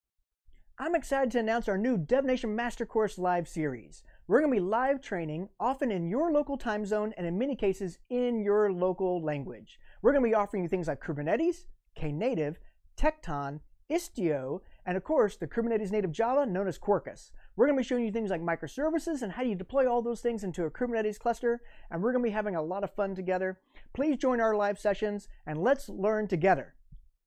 Join us for DevNation Deep Dives
Join Red Hat Developer weekly for DevNation Deep Dives. These one-hour live streams will cover topics including Kubernetes, Istio, Tekton, Quarkus, Knative and so much more. Check our page for upcoming courses and invite your friends to these dynamic and fun learning sessions.